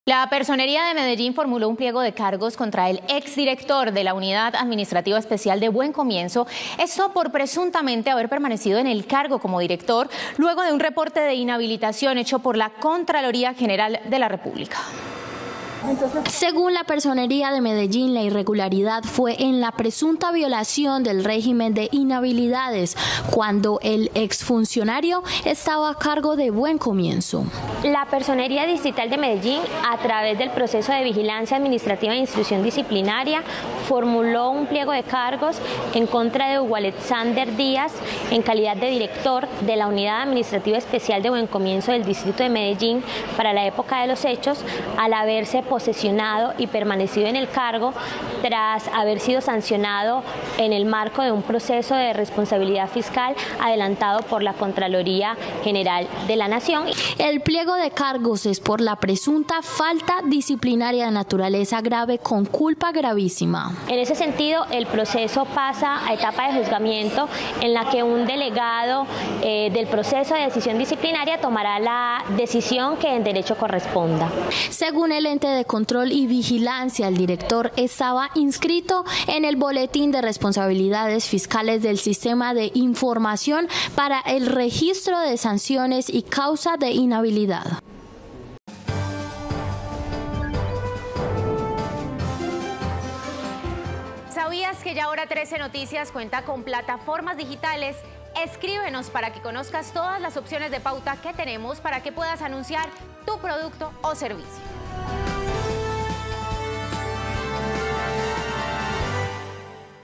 Personería de Medellín formuló pliego de cargos contra ex director administrativo de Buen Comienzo
El pliego de cargos es por la presunta falta disciplinaria de naturaleza grave con culpa gravísima.